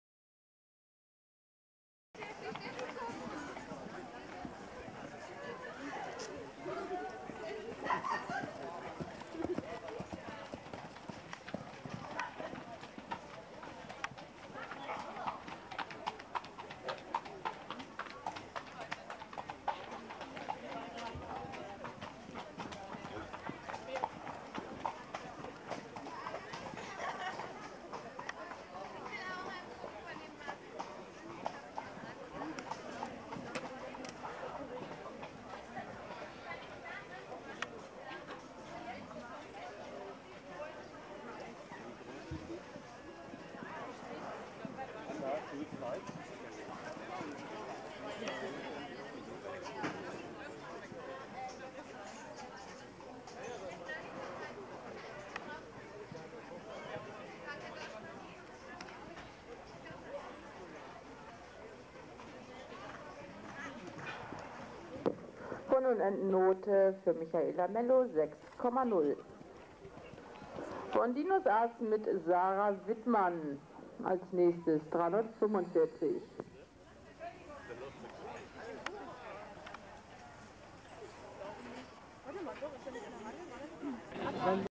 Max und ich in Einbeck
2005: kleines Springen - leider Stil, wo man als No-Name eh keinen Platz macht!